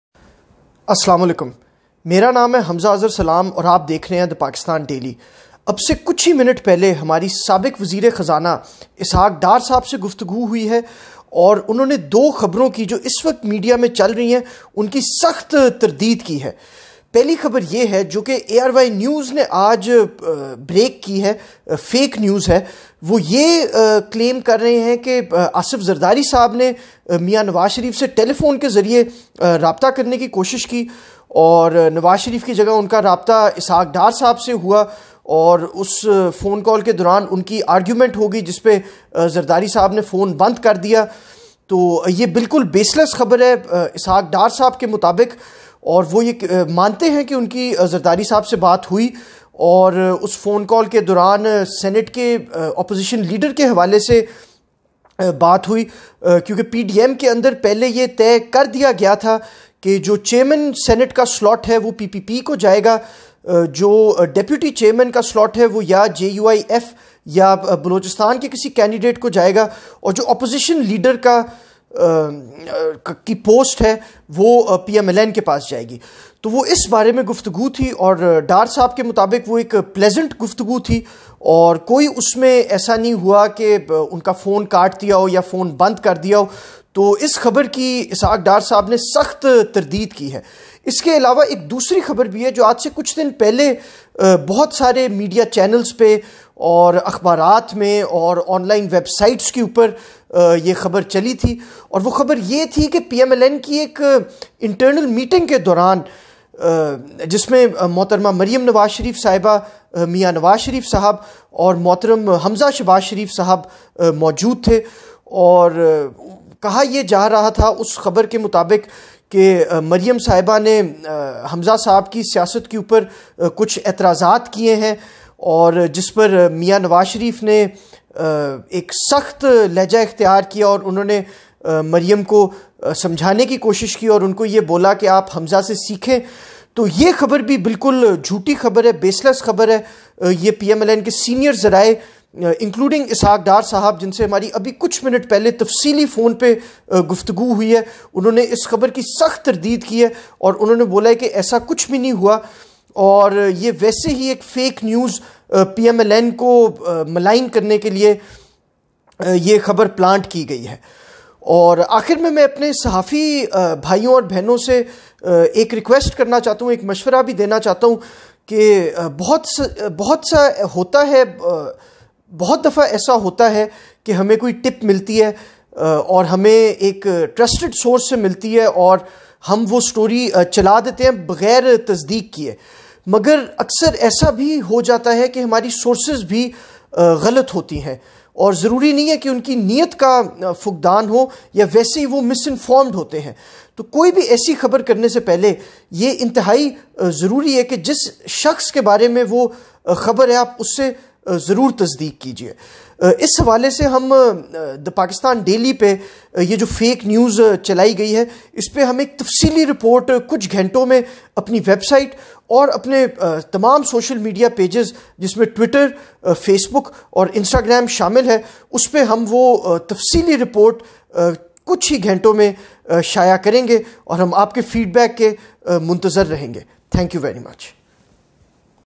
Reality behind Asif Zardari - Ishaq Dar fight
After speaking to Ishaq Dar, Hamza Azhar Salam tells the reality of the alleged fight between Asif Zardari and Ishaq Dar.